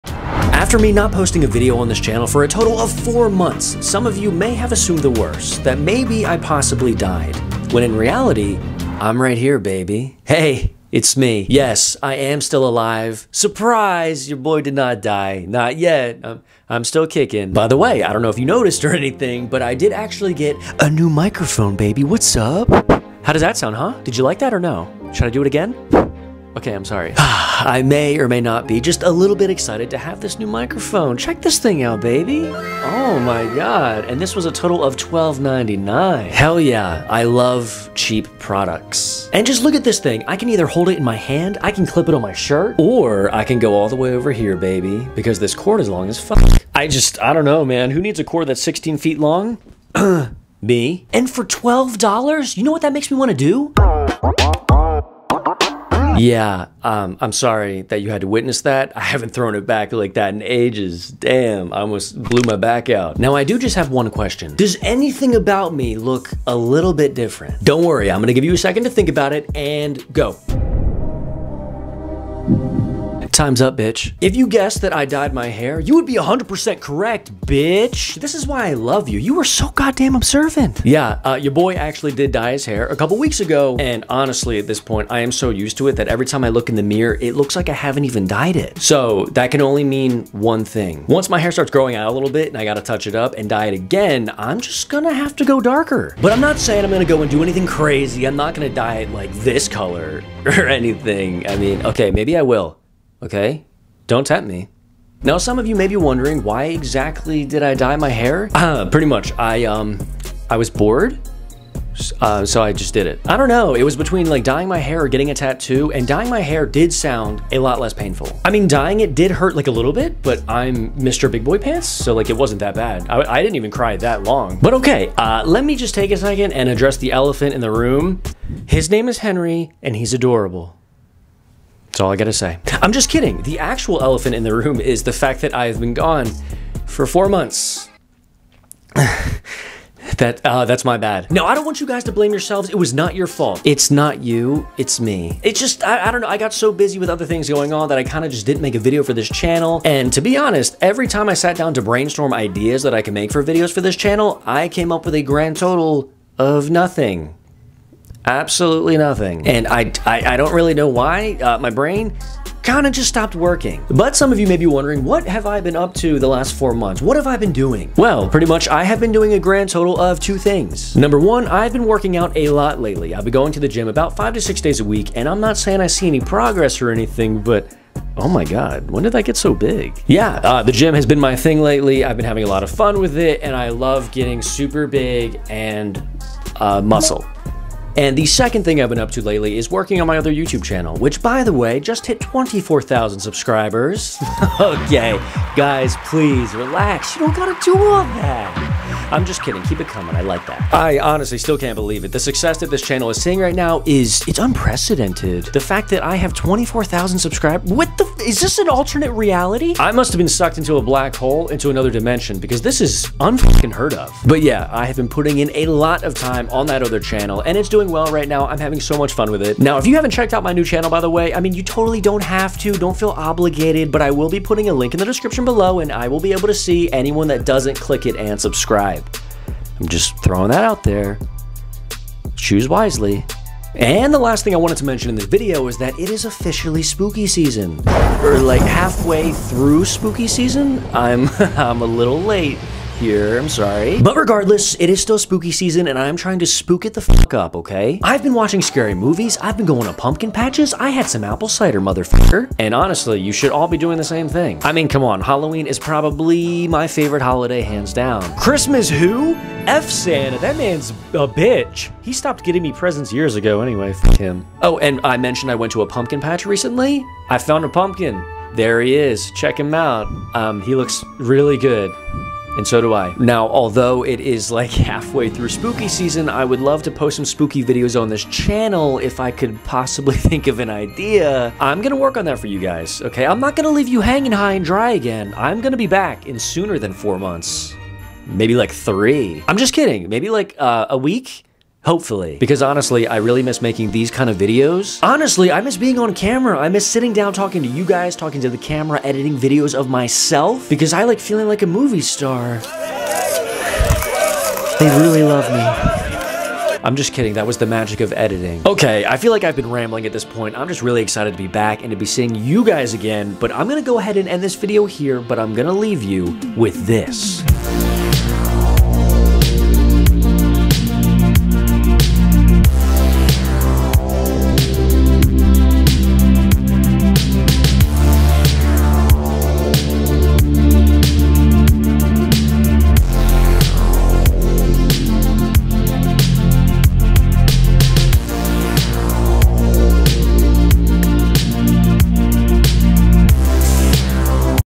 Surprise... I'm still alive
After disappearing from this channel for 4 whole months I'm finally back to let you all know that... I'm still alive. Unfortunately my video idea brainstorming sessions did not pay off at all over the last 4 months which is the main reason why I haven't been posting. But I'm back & better than ever & HOPEFULLY gonna see you again in less than 4 months.

FLASH WARNING @ 5:02

At the time I'm uploading this video we're at 5,306 subscribers. Let's get that 100,000 plaque baby.